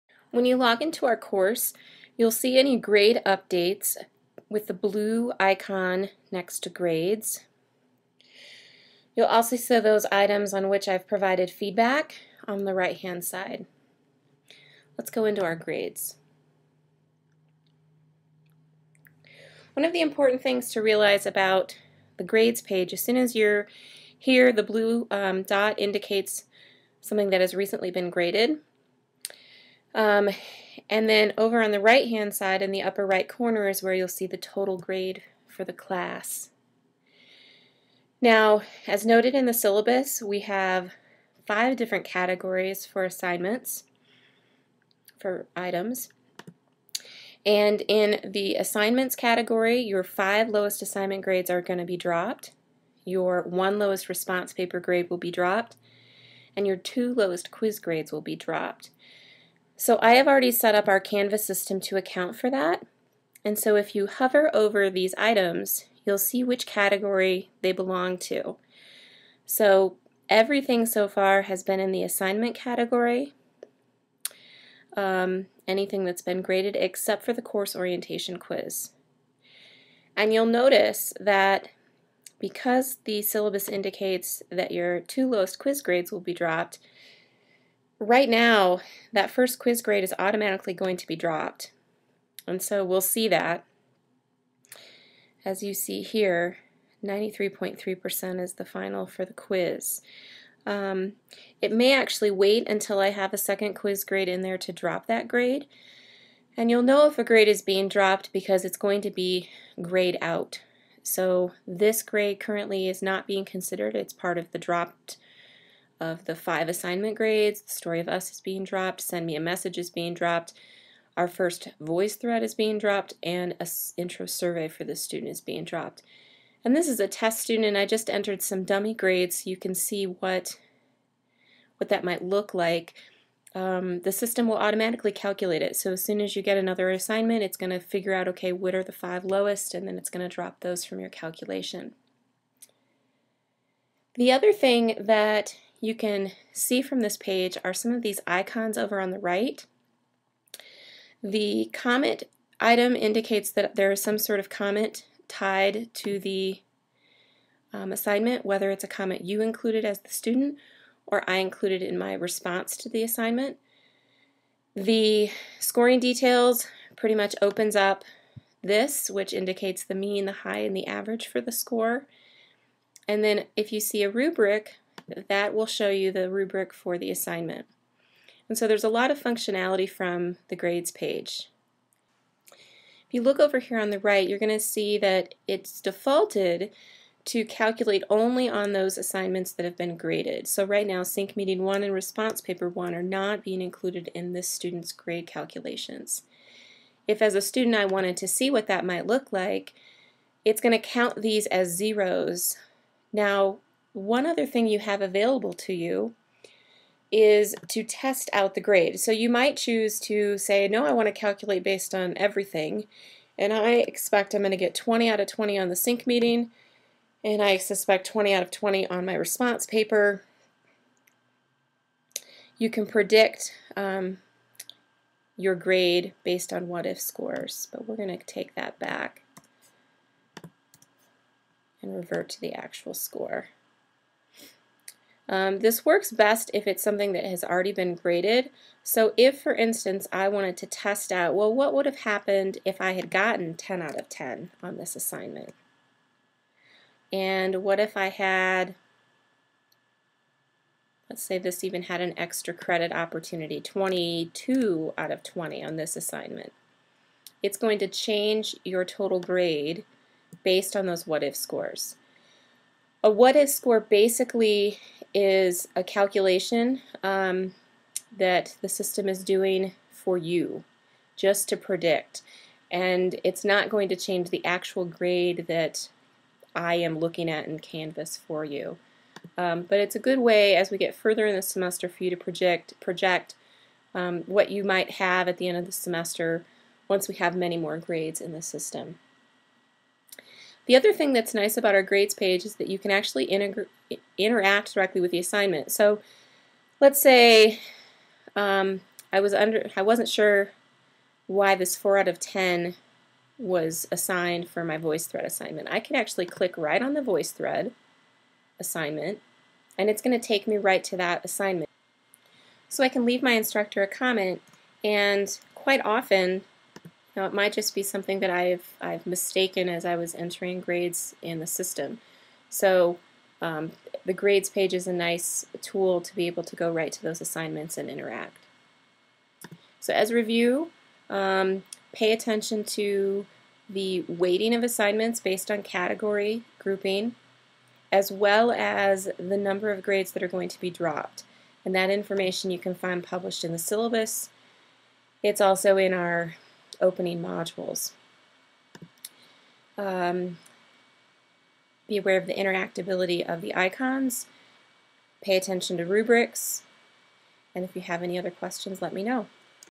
Grades_LIT115.mp4
In this video I explain how to interpret and interact with the grades page in our Intro to Lit Canvas course.